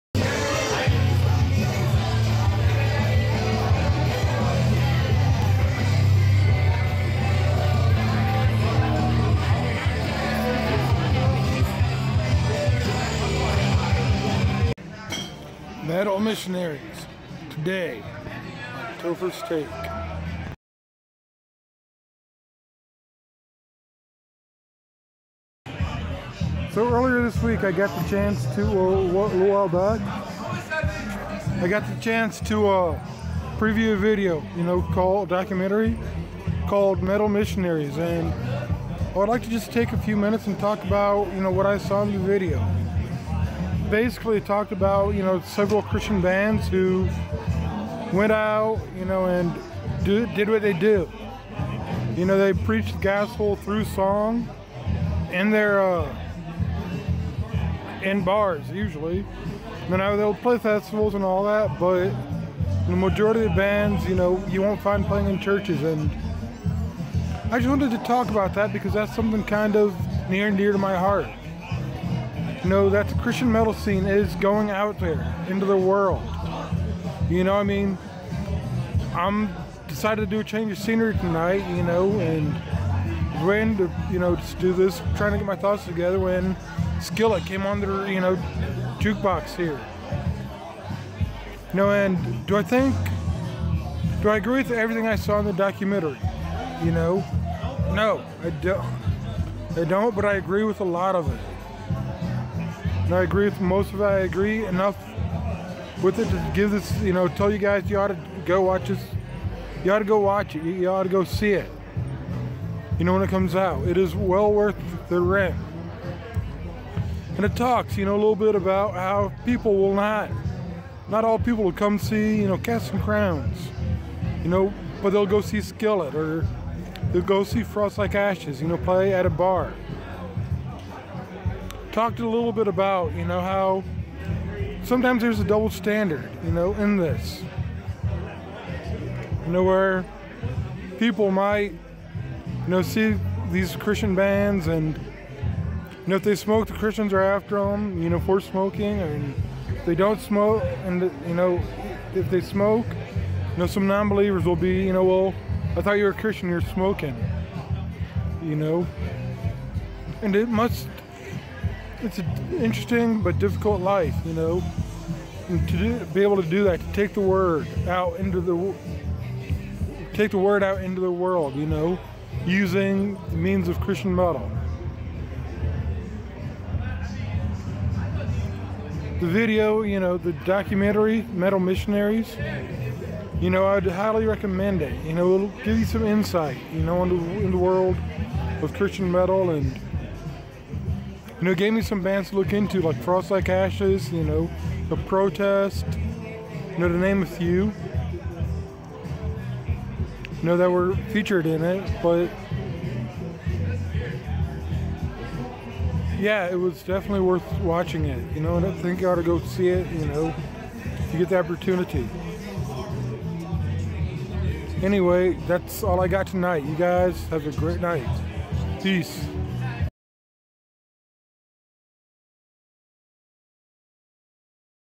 Tofer's Take: Metal Missionaries
I got the chance to preview this documentary. Just a few thoughts on it. Feel free to check it out.